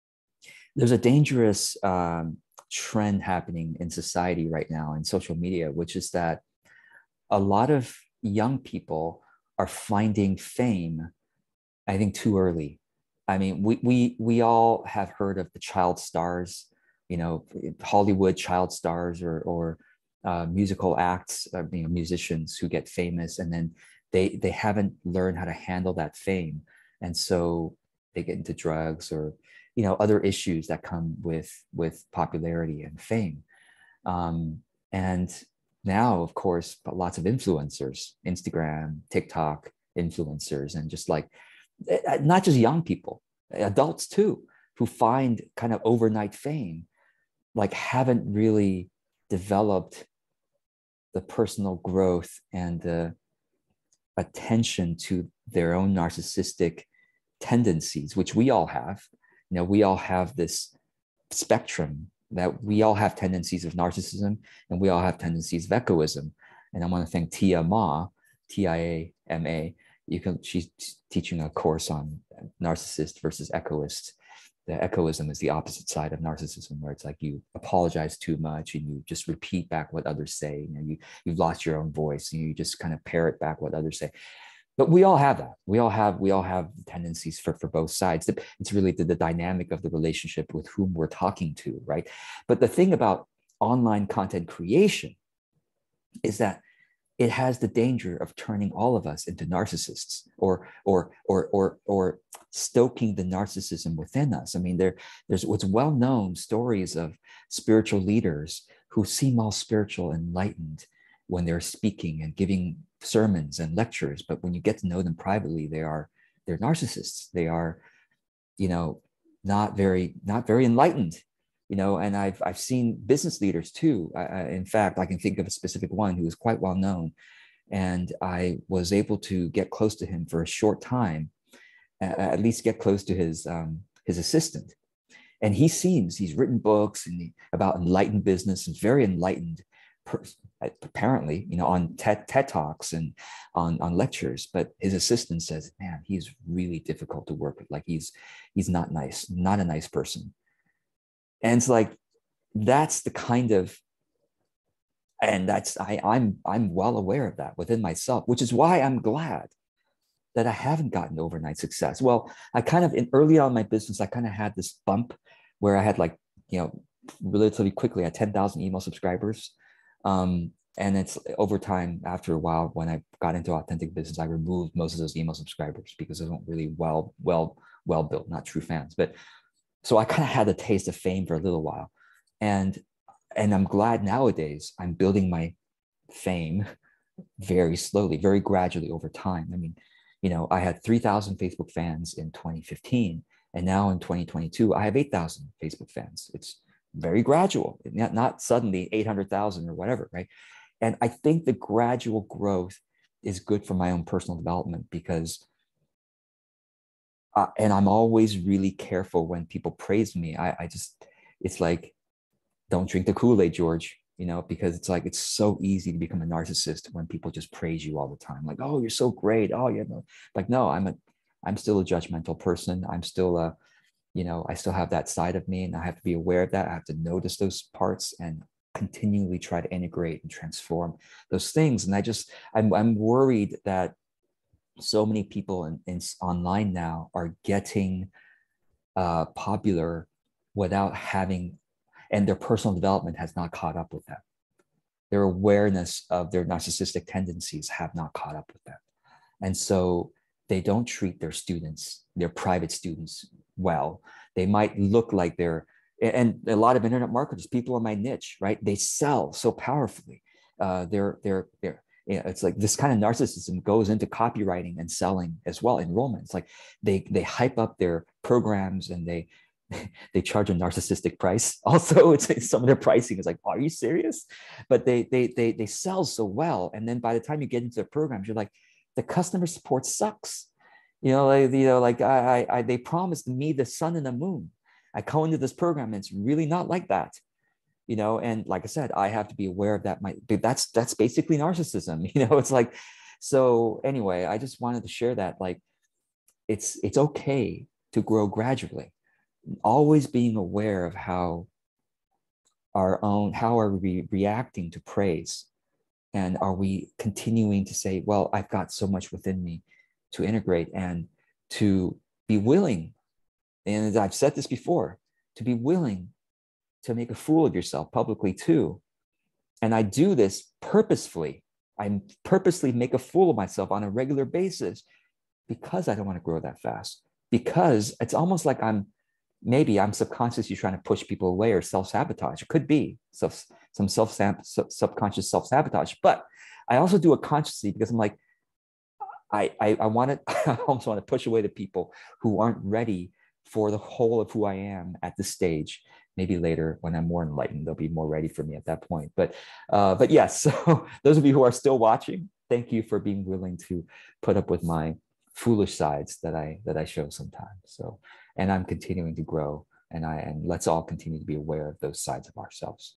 Influencer danger -- gaining following quickly, narcissism, not having integrity for fame/popularity
This is a segment from my Monthly Q&A call. You get access to 2 months of these live calls when you sign up for any of my courses